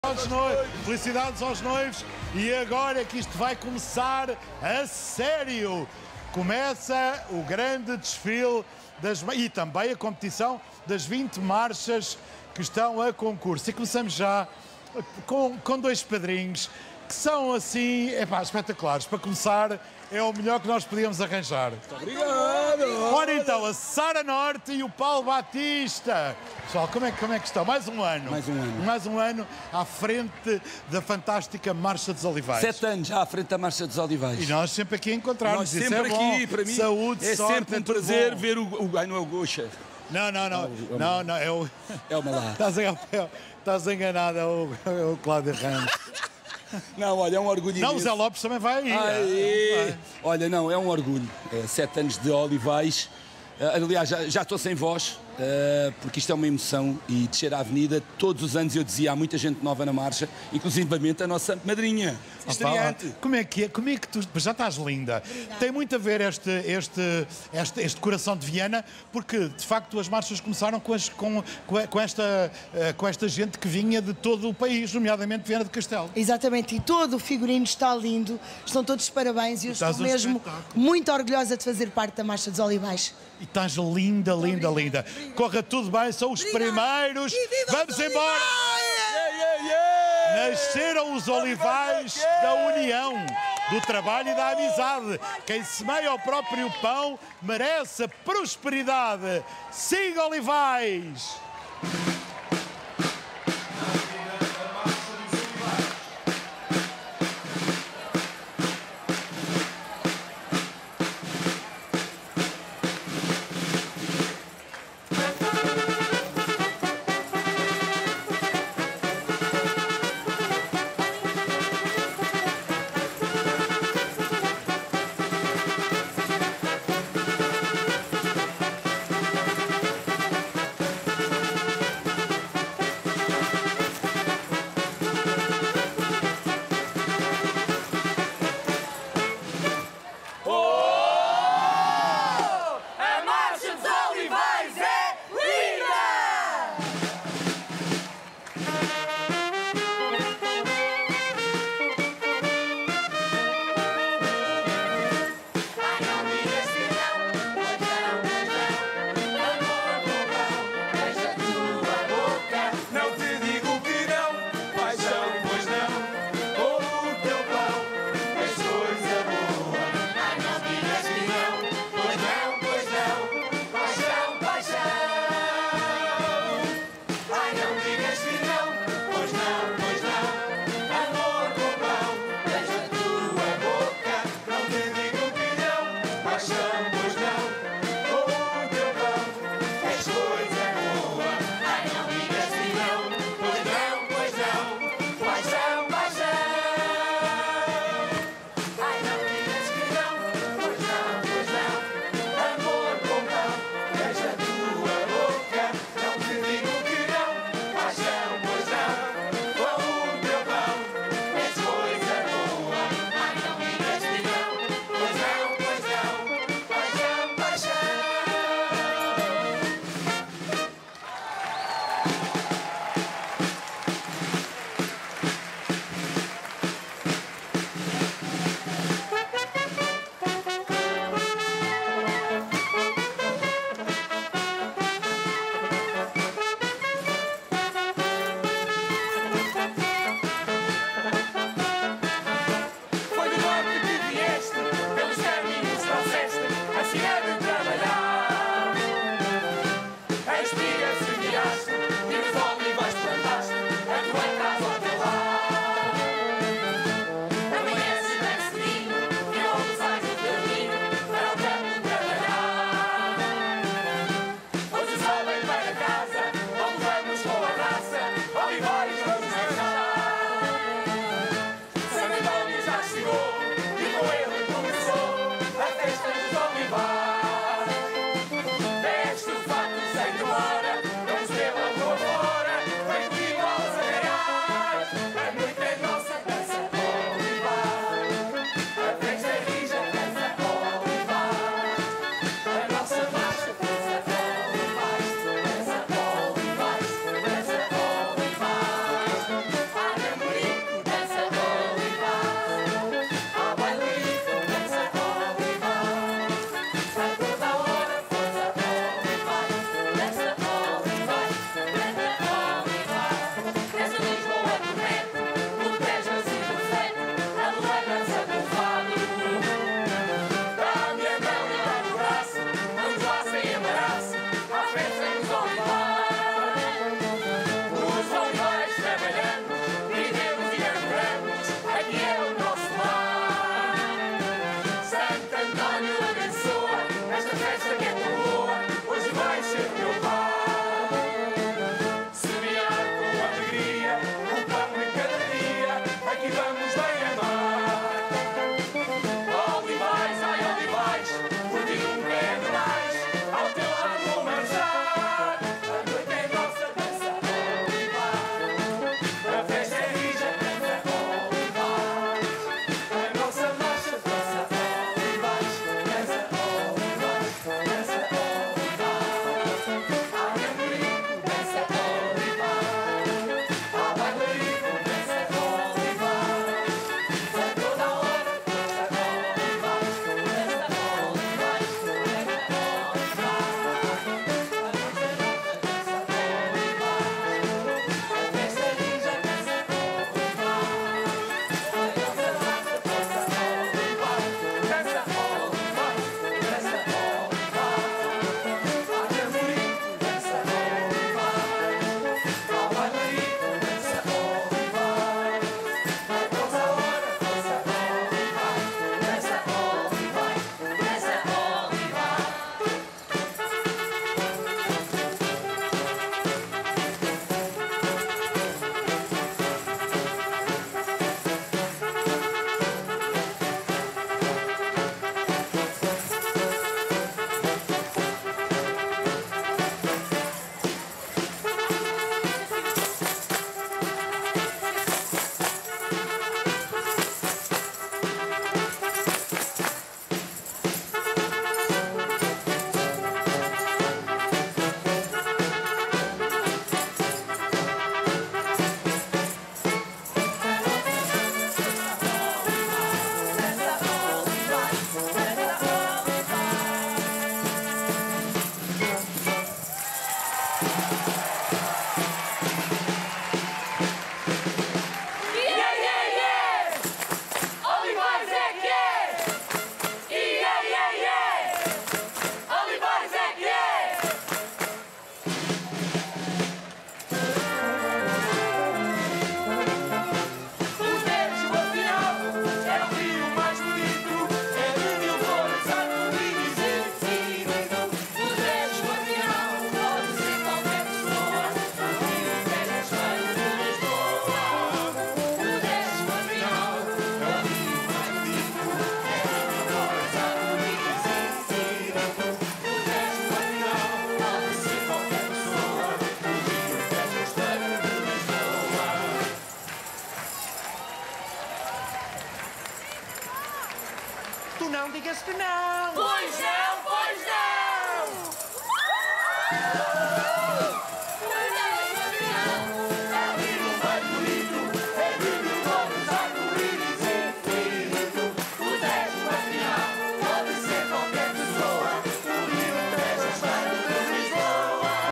MARCHA DOS OLIVAIS 2024 - AVENIDA
!3° Lugar das Marchas Populares de Lisboa 2024: 🥁

Tema - 'Olivais, o pão de cada dia'

Padrinhos: Paulo Battista e Sara Norte
Ensaiadora: Sara Brandão
Figurinista: Brandão & Barros
Cenógrafo: Brandão & Barros e Carlos Ferreira

Transmissão RTP1 🎥

Marchas Populares ✍️